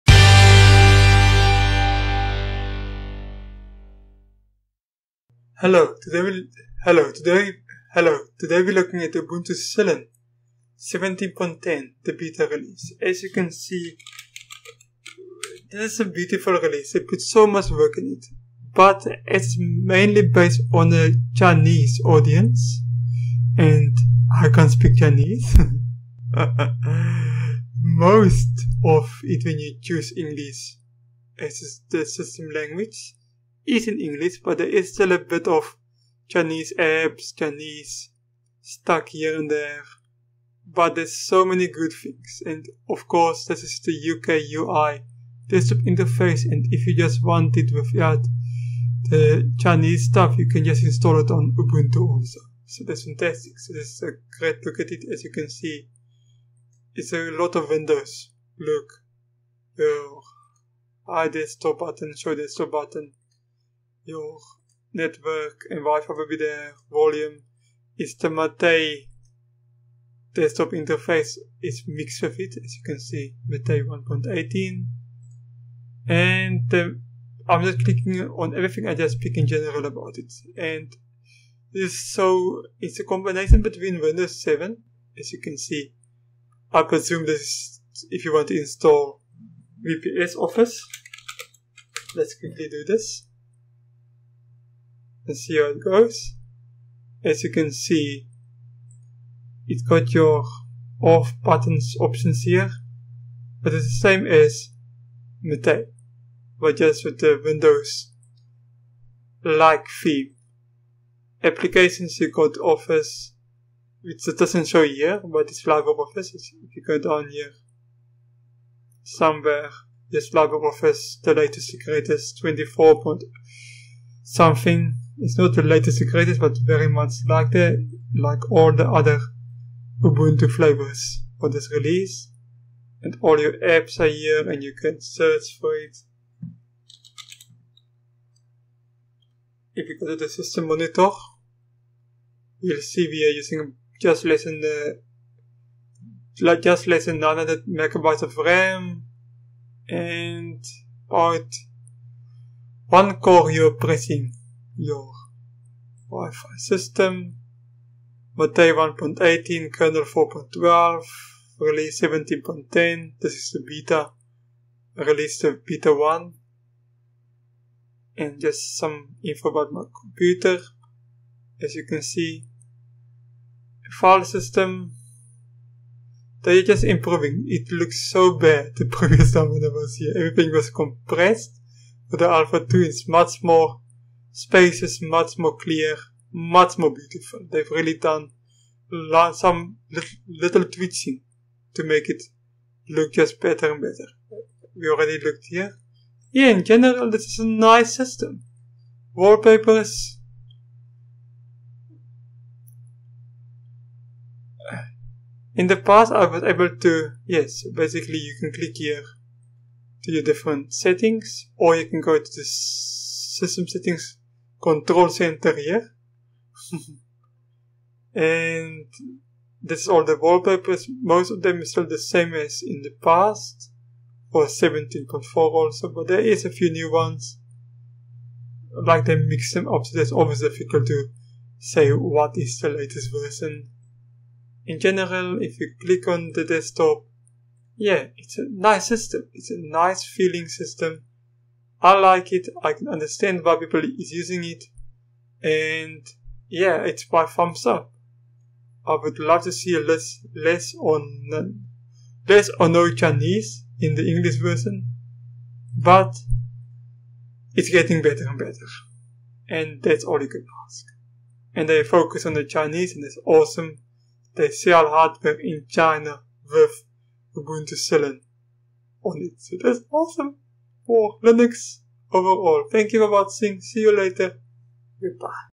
Ubunu Kylin 17.10 Beta 1
In this distro preview we are looking at Ubuntu Kylin 17.10 Beta 1.